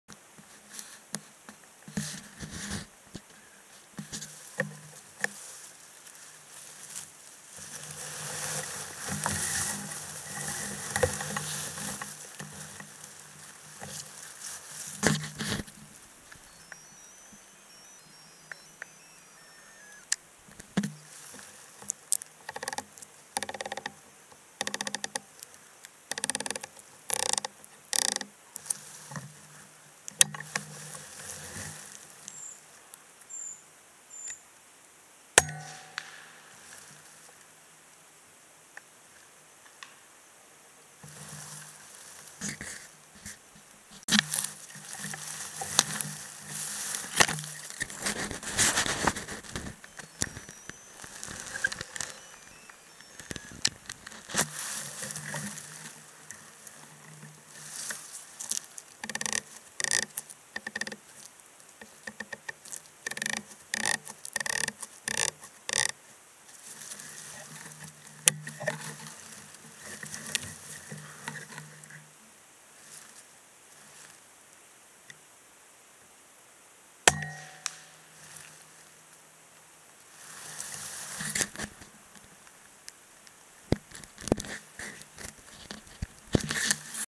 Pari pudotusta
Pienoiskiväärillä kyyhkyn kyttäystä elokuun lopulta.